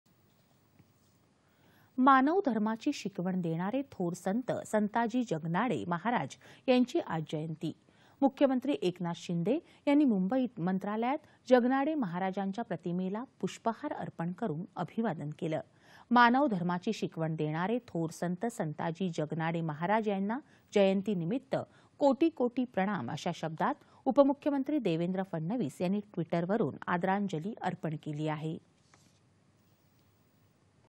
संत संताजी जगनाडे महाराज यांची जयंती
दि.०८.१२.२०२२ :
     मानव धर्माची शिकवण देणारे थोर संत संताजी जगनाडे महाराज यांची आज जयंती. मुख्यमंत्री एकनाथ शिंदे यांनी मुंबईत मंत्रालयात जगनाडे महाराजांच्या प्रतिमेला पुष्पहार अर्पण करुन अभिवादन केलं.
      मानव धर्माची शिकवण देणारे थोर संत संताजी जगनाडे महाराज यांना जयंतीनिमित्त कोटी कोटी प्रणाम, अशा शब्दांत उपमुख्यमंत्री देवेंद्र फडणवीस यांनी ट्विटरवरुन आदरांजली अर्पण केली आहे.